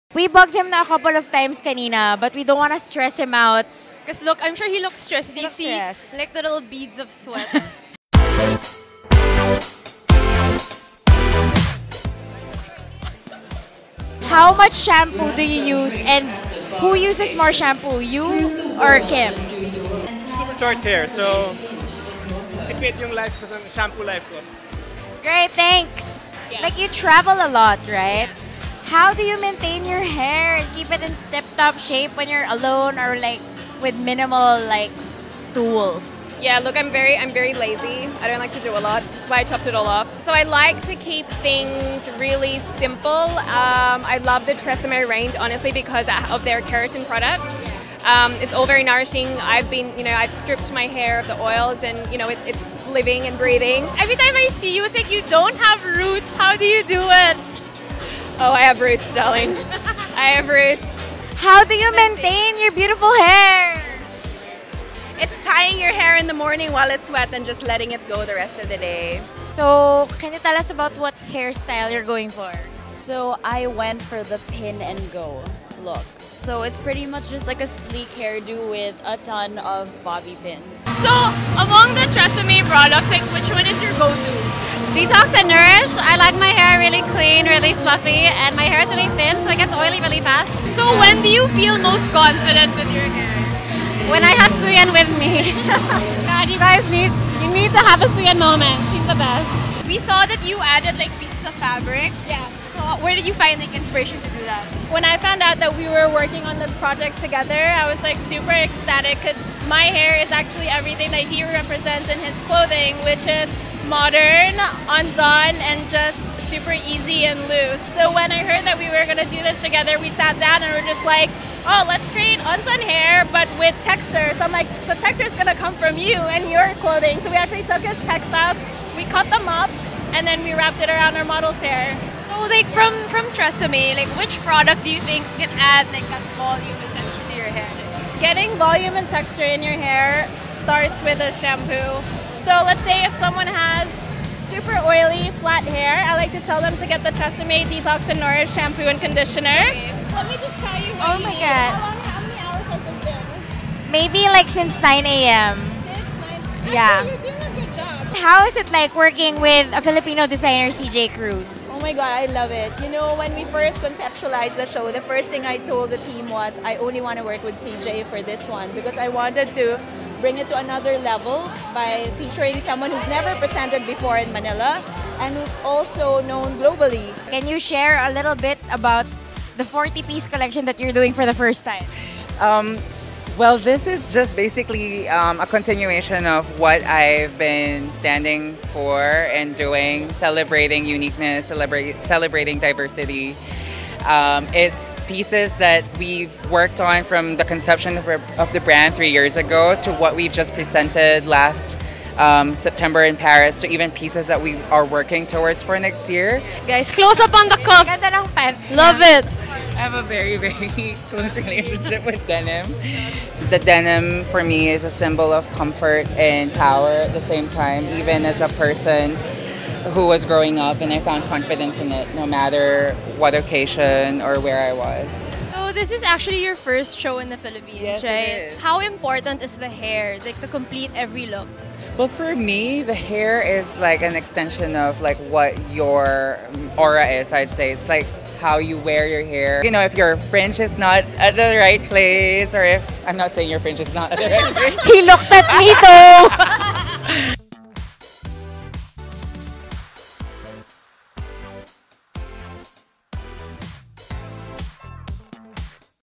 Runway Ready with TRESemmé | Preen Online
Carl Jan Cruz and his 40-piece collection's first runway show in Manila was presented by TRESemmé. Watch us learn more about how seriously men and women take haircare and how a certain hairstyle can complete your look!

Video by Samantha Ong and Javier Lobregat
Audio by Jonas Timbreza and Karl Angay